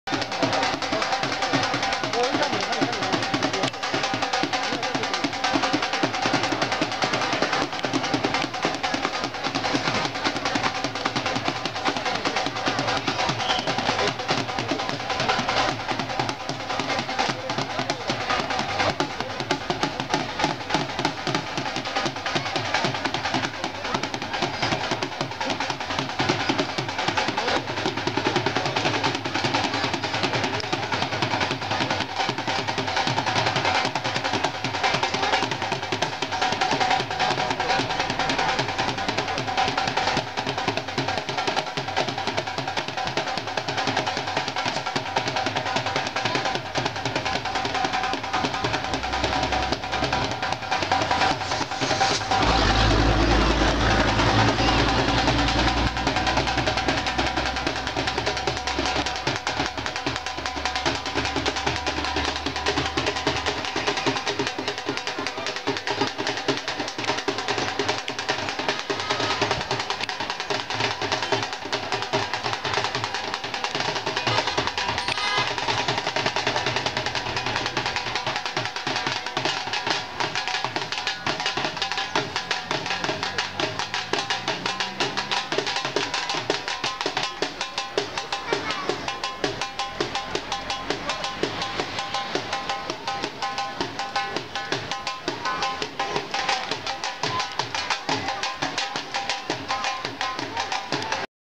Kushti, हिमाचल में कुश्ती का आनंद।
Kushti, A means of emphasis trial. At its peak in festivals and fairs.